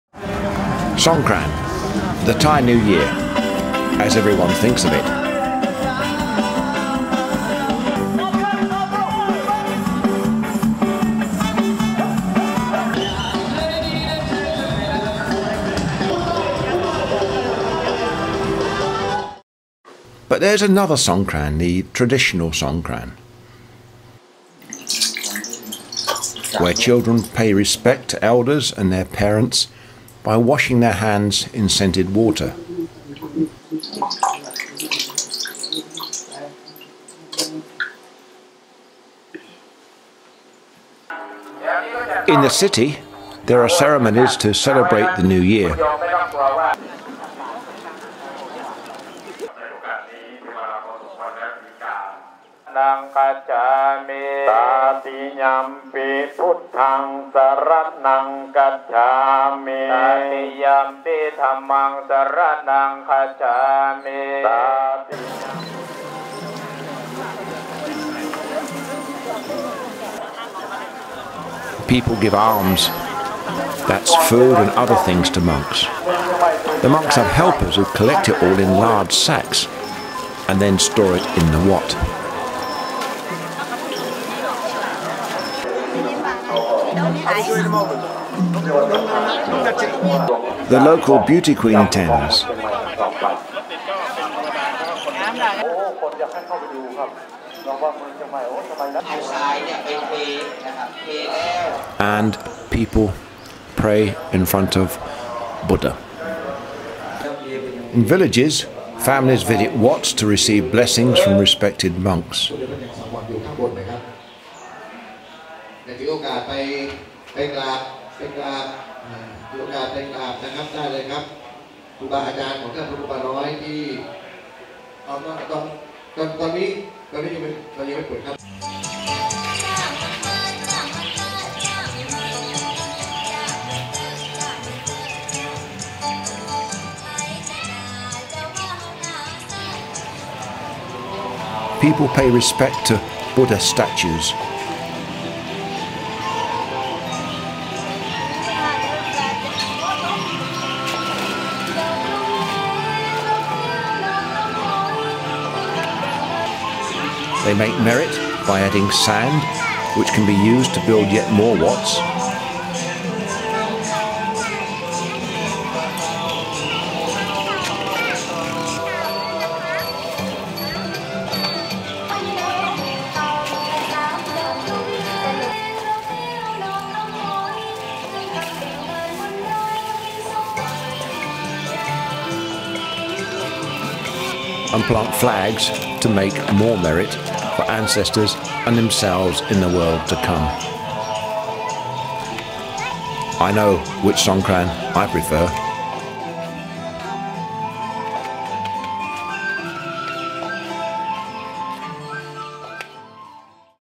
Traditional Songkran in Chiang Mai 2015
Everyone thinks of Songkran as water fights and soakings. But there is a more traditional side. The way it is celebrated in Wats  and villages. This is a quick snapshot of 2015